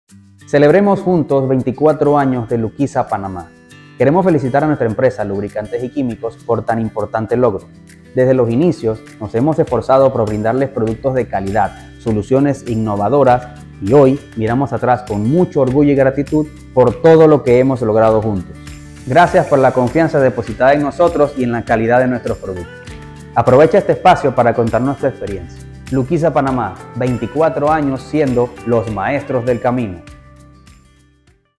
24 años siendo los maestros del camino | Luquisa Panamá
¡LUQUISA ESTÁ DE FIESTA!
¡24 años siendo los maestros del camino! 🎉🥳

Agradecemos muy especialmente a todo nuestro equipo humano (colaboradores), por su esfuerzo y empeño; a nuestros clientes y proveedores, por su compromiso y confianza en la calidad de nuestros productos. 🙌🏼

¡Cuéntanos tu experiencia en el uso de nuestros productos y recomiéndalo a otras personas!